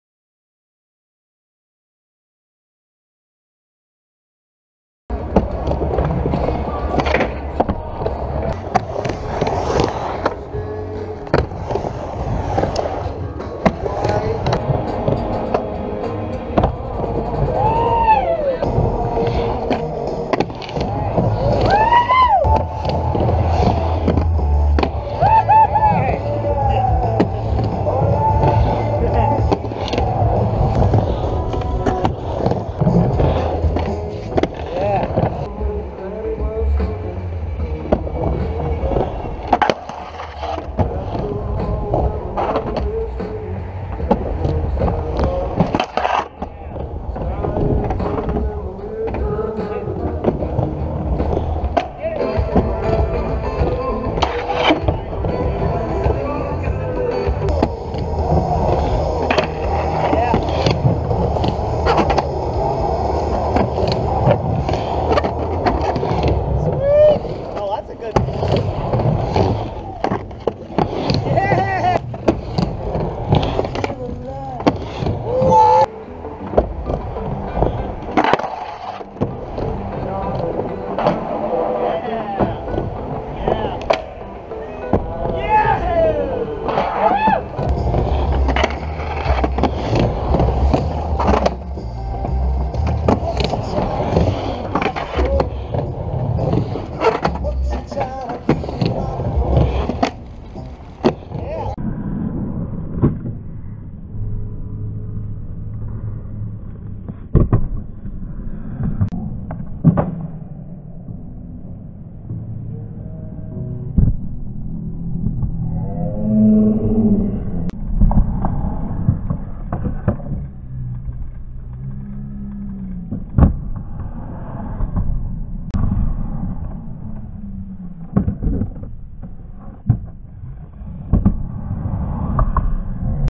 Paul and Scott Vert Session 04March2018
Paul Gierow and Scott Hughston - Insanity Skatepark in Madison, AL - 04 March 2018.  Both of us were born in 1963 and have been skating since the mid-1970s.